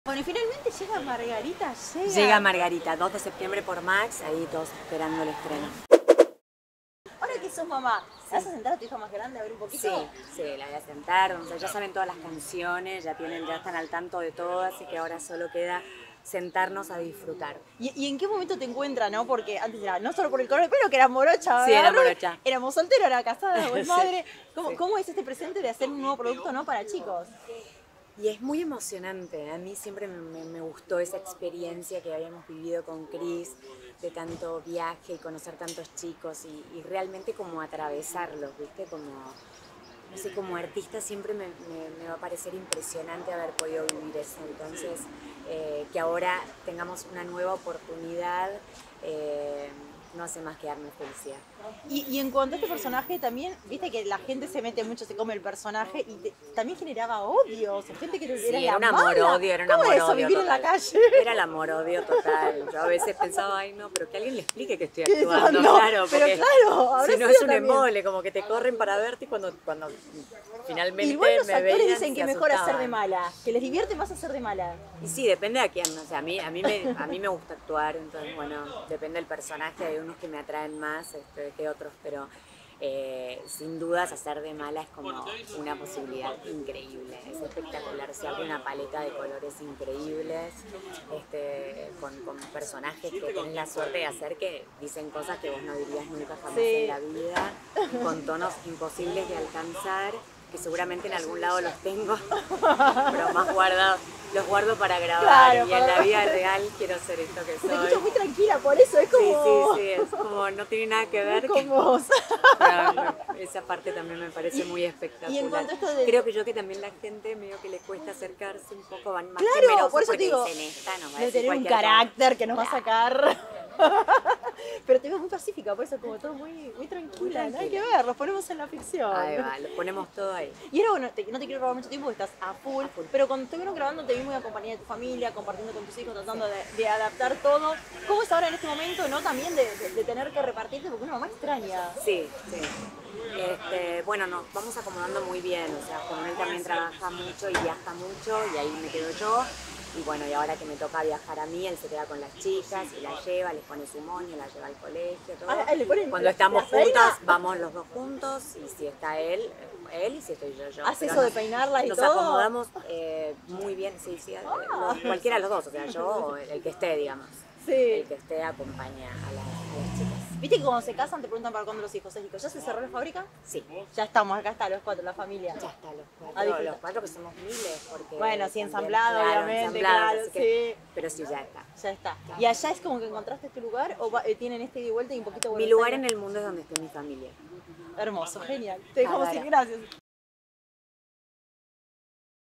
ISABEL MACEDO y su confesión sobre Floricienta: "HACER DE MALA ES UNA OPORTUNIDAD INCREÍBLE..."
Isabel Macedo se sumó a 3MinutosMas y nos contó toda la intimidad del estreno de "Margarita" y cómo se encuentra personalmente en esta etapa

🔗Mirá la nota completa en nuestro link en el perfil o en paparazzi.com.ar